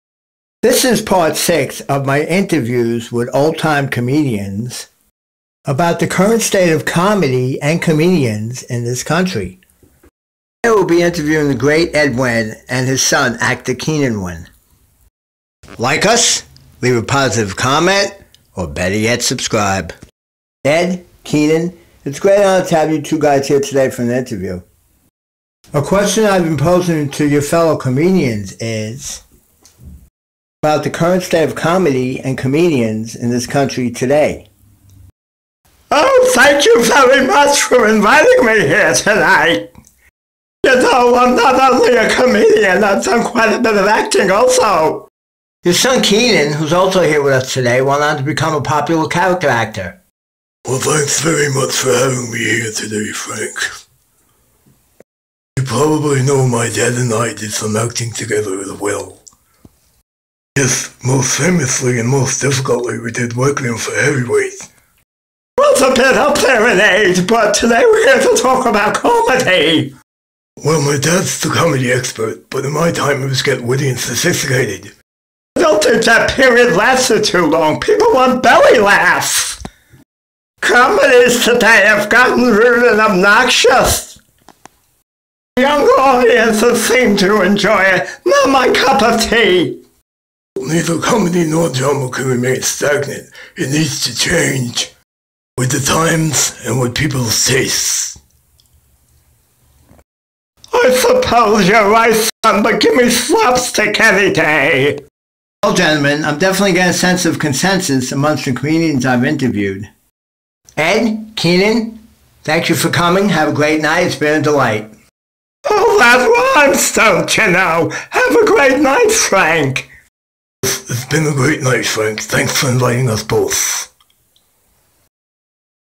Interview W/Ed&Keenan Wynn:Impressions:Frank Bruno
Part 6 of Master Impressionist Frank Bruno's series of interviews with old-time comedians. If you like the series please share it.